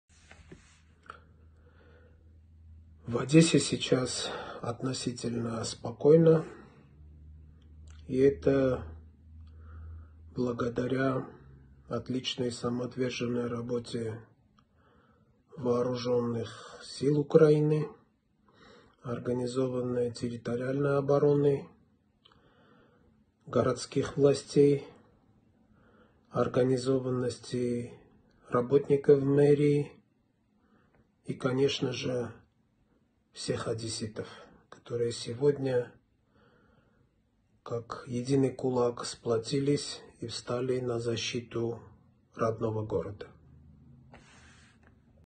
Одесити згуртувалися, як єдиний кулак
Глава вірменської громади в Одесі Тигран Арутюнян: «В Одесі зараз відносно спокійно – завдяки самовідданій роботі ЗСУ, територіальної оборони, міської влади, організованості працівників мерії та, звичайно, всіх одеситів, які сьогодні згуртувалися, як єдиний кулак, і стали на захист рідного міста».